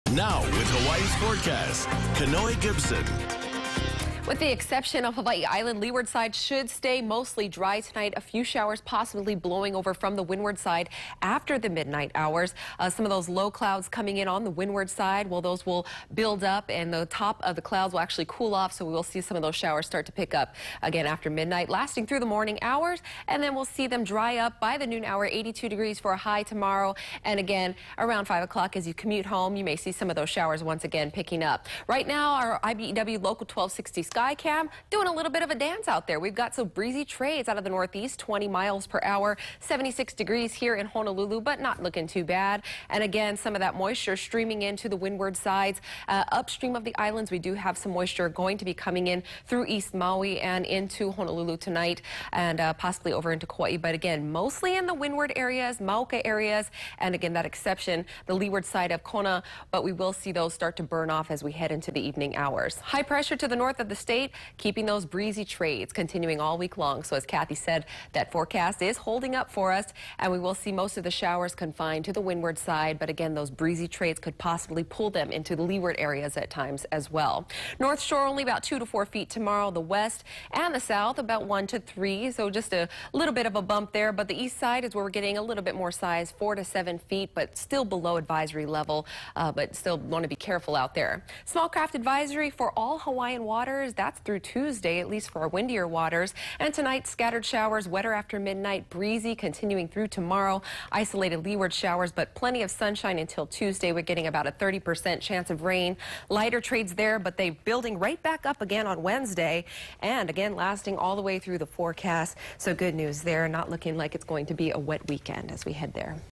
KHON2 Weather Forecast
Seasonable and locally breezy trade winds will prevail through the upcoming week, delivering passing clouds and showers.